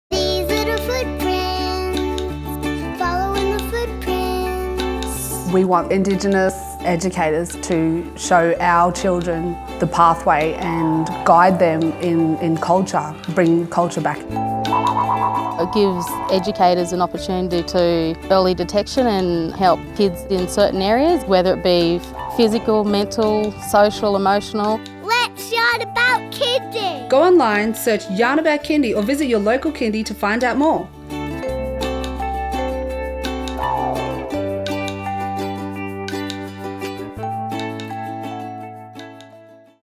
Aboriginal and Torres Strait Islander role models
Shandell Washington and Caitlyn-Maree Spaul said they are passionate about their work and that more people, particularly Aboriginal and Torres Strait Islander people, should consider a career in early childhood. Read more of their stories on The Early Years Count